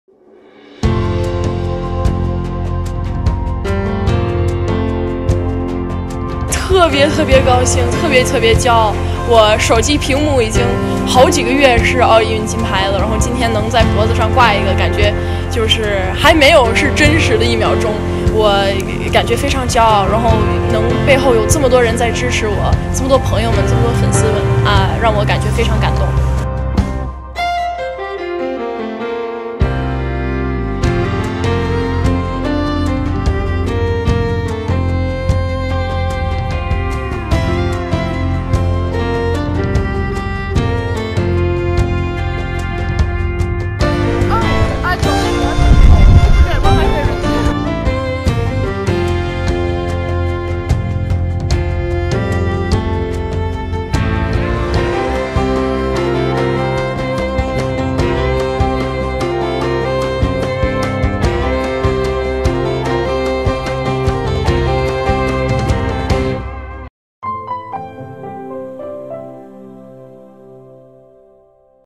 一分钟回顾谷爱凌在北京冬奥会的高光时刻
2022年2月18日，北京冬奥会自由式滑雪女子U型场地技巧决赛中，中国队选手谷爱凌一举夺冠，这也是她在此次冬奥会获得的第二枚金牌。2月8日在自由式滑雪女子大跳台决赛中，谷爱凌获得金牌。2月15日在自由式滑雪女子坡面障碍技巧比赛中，谷爱凌为中国队再添一枚银牌。